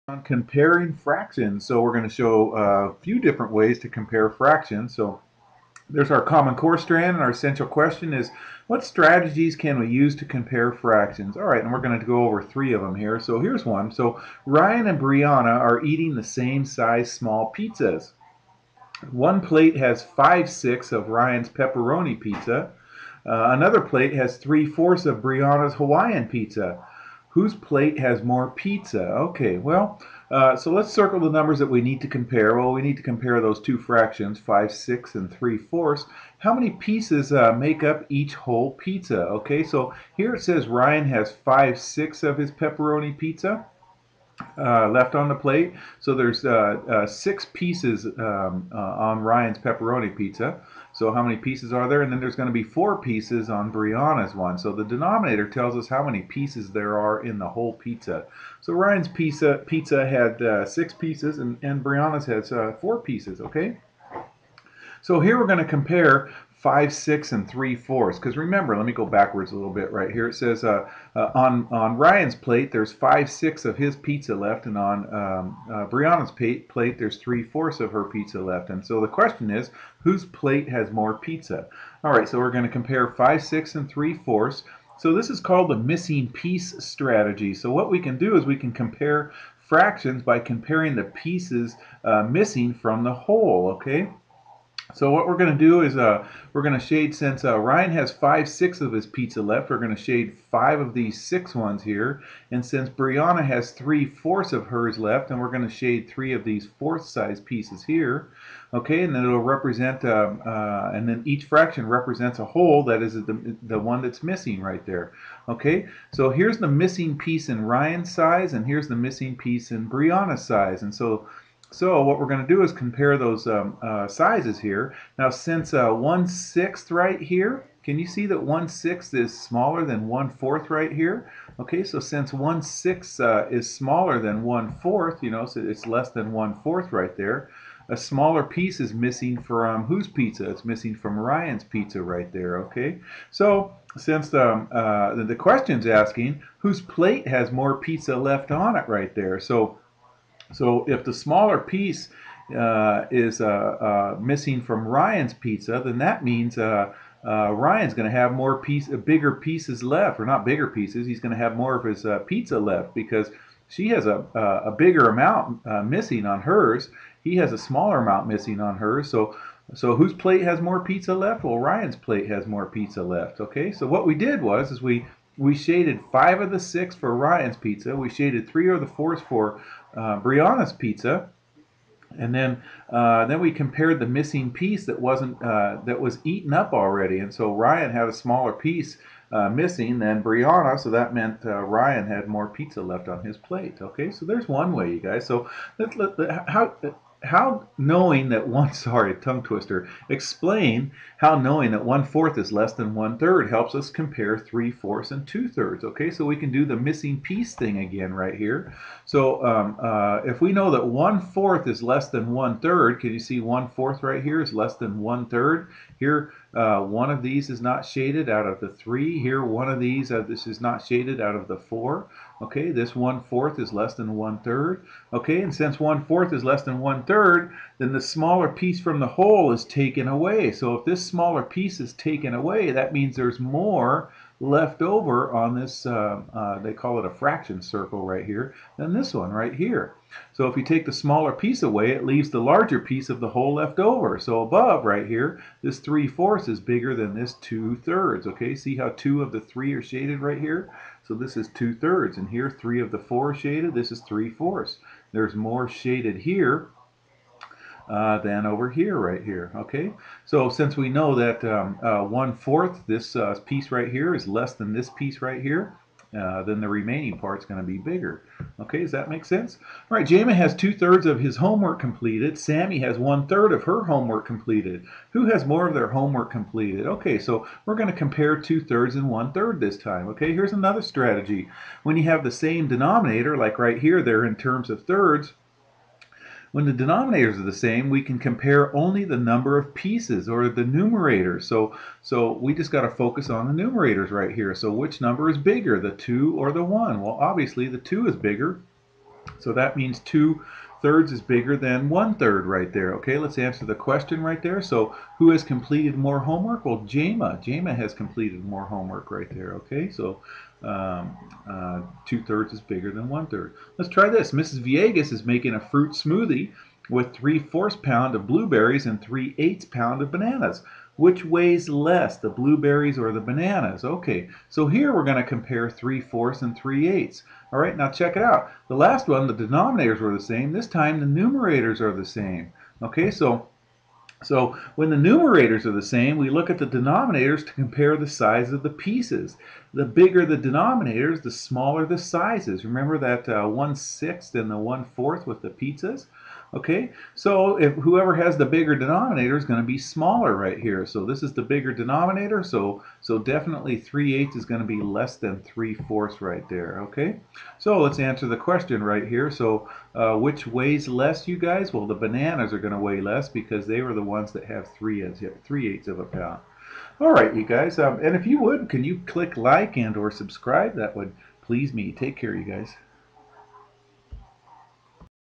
Compare Fractions - Lesson 9.4
This lesson compares fractions using the missing piece strategy, fractions that have the same numerators, and fractions that have the same denominator. Go math!